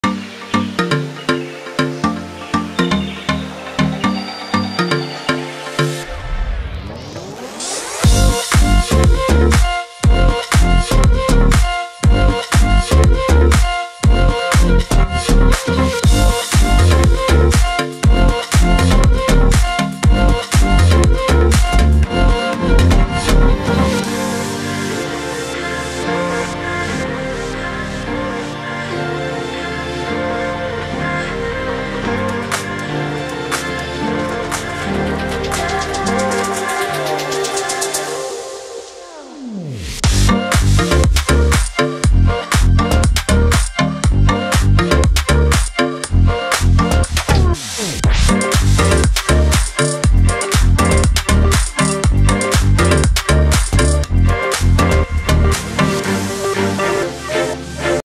How to cut a Shih Tzu - GroomerOK
Tomorrow you will see an interesting video how I groom a shih tzu. This dog doesn’t like to comb his hair.  And so his coat always has tight knots.
My videos are released twice a week on my channel.  Friday - preview , Saturday - full video of dog haircuts.